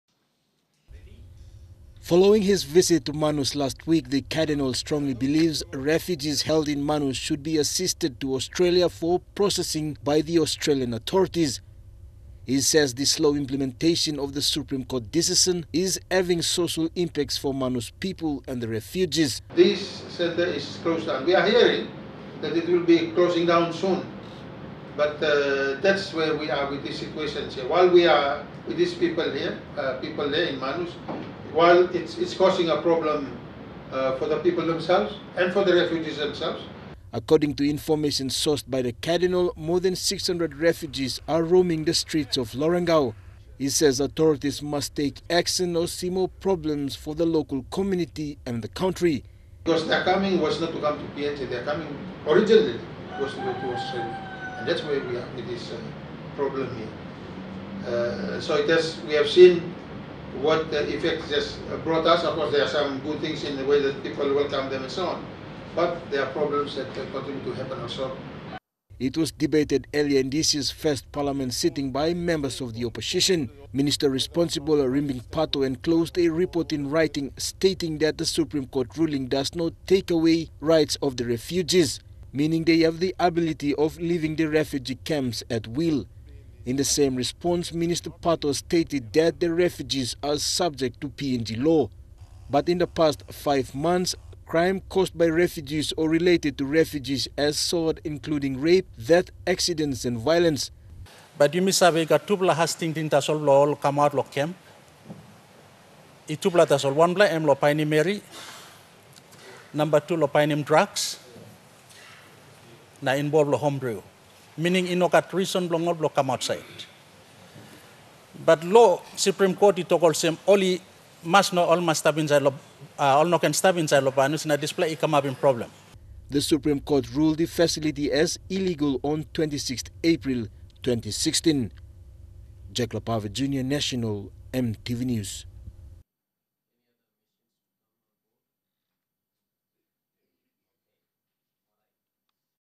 Cardinal Ribat says refugees must be transferred to Australia for processing
Cardinal Sir John Ribat says refugees in Manus must be transfered to Australia for processing.

Cardinal Ribat questioned why the Supreme Court decision has not taken action.

He made this statement following his recent visit to Manus.